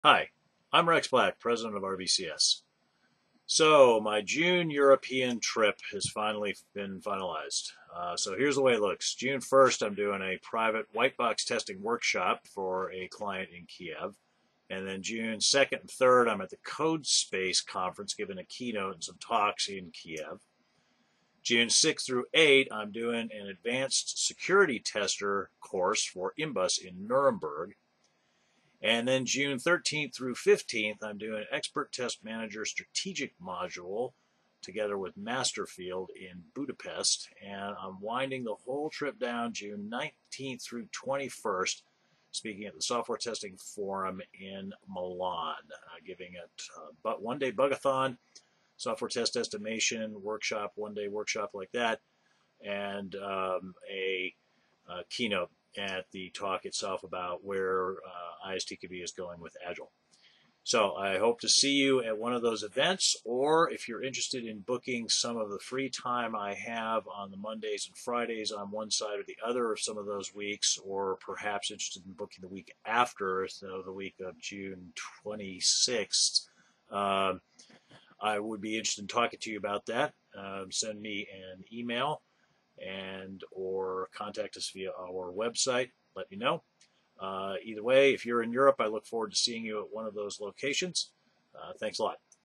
RBCS: Rex Black Europe Tour June 2017
Yes, I am coming to Europe in June, and you can meet me at one of the following events:
June 1: White-box testing tutorial through Luxoft and Code Space as part of the Testing Stage 2017 conference
June 19-21: Black-box Bug-a-thon and Software Test Estimation at the Software Testing Forum